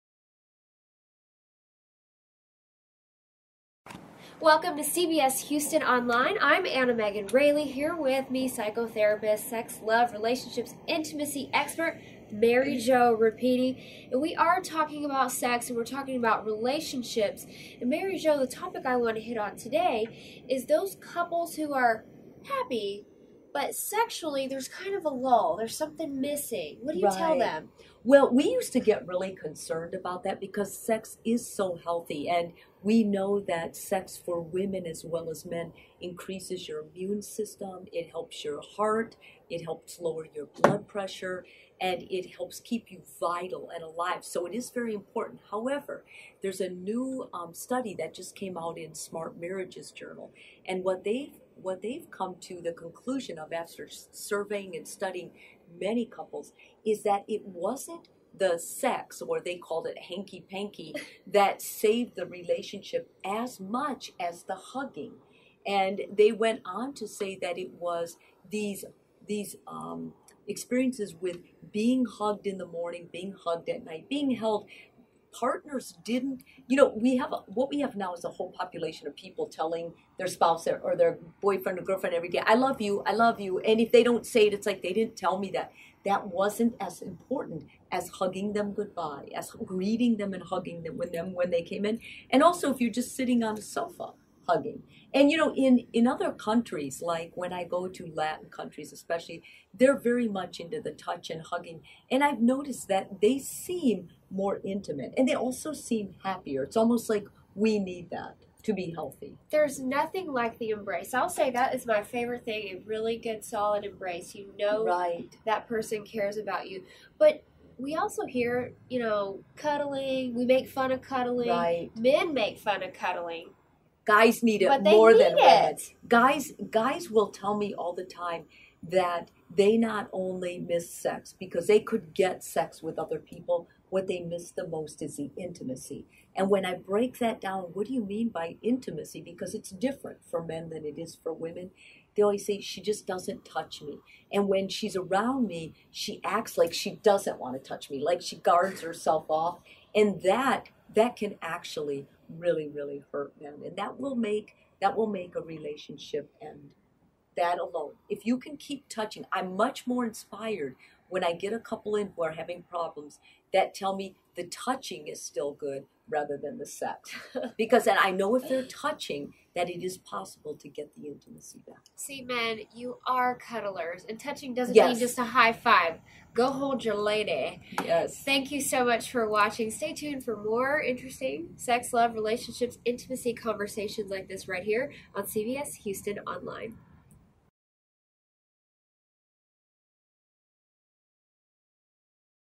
Sex, Love and Relationships.avi
Anna-Megan Raley interviews Mary Jo Rapini about Sex, Love and Relationships.